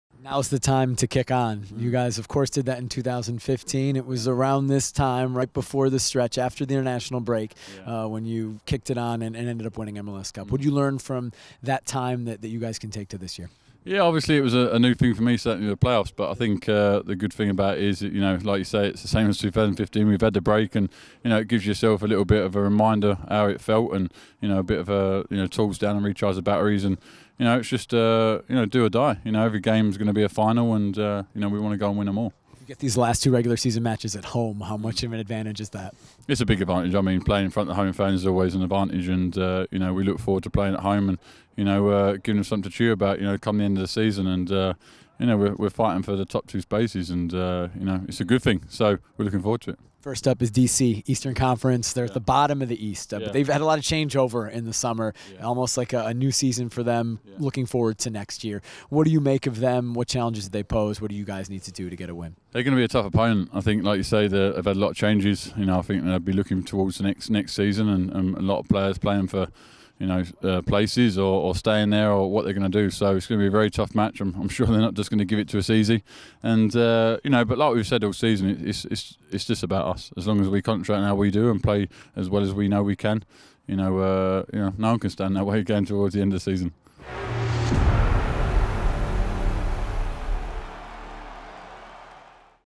Liam Ridgewell on facing D.C. United | Timbers Training
Timbers defender Liam Ridgewell talks about taking on D.C. United this weekend and building momentum for the end of the season.